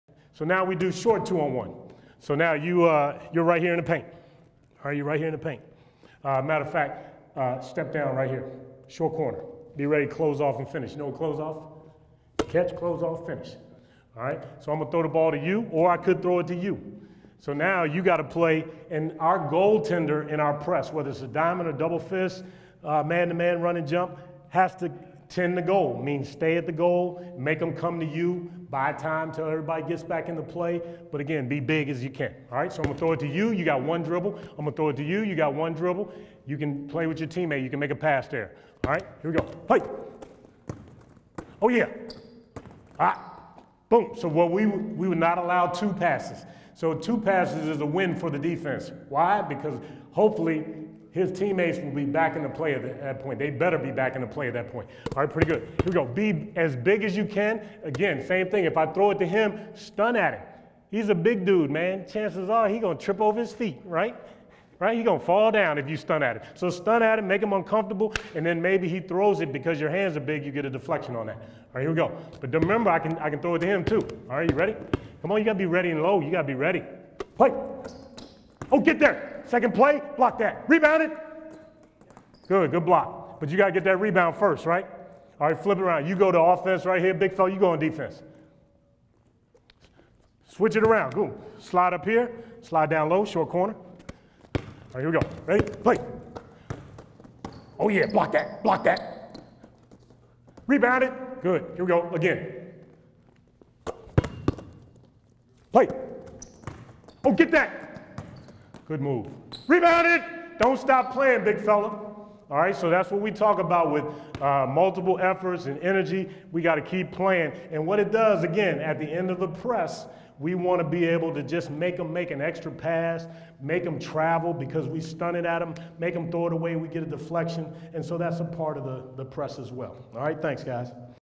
Mike Jones' "Short 2-on-1" Basketball Outnumbered Drill!
with Mike Jones,
Radford University Head Coach;
2018 Big South Coach of the Year;
2018 Big South Tournament champions;
2011 NCAA Final Four (at VCU as Assistant Coach)